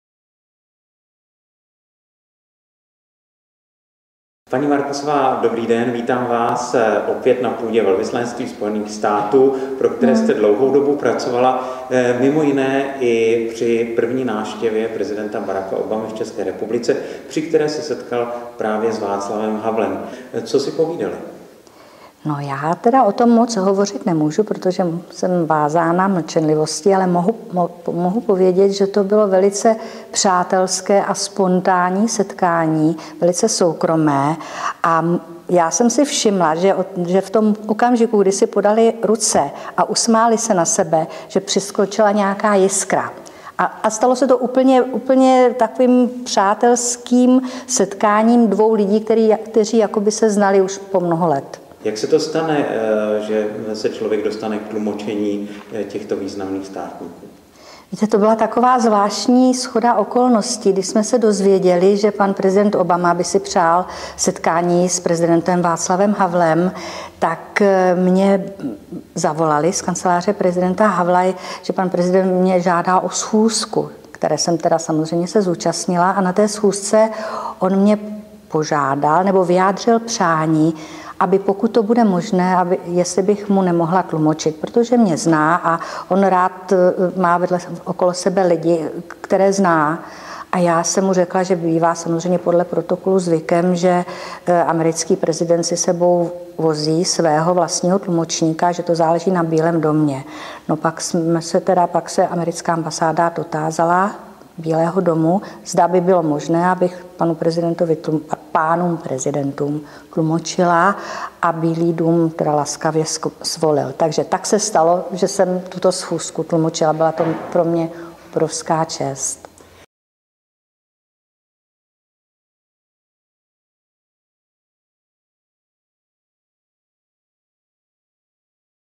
Setkání Václava Havla s prezidentem USA Barackem Obamou
Helena Markusová, která pracovala na Velvyslanectví USA v Praze,
tlumočila setkání Václava Havla s americkým prezidentem Barackem Obamou
6. dubna 2009.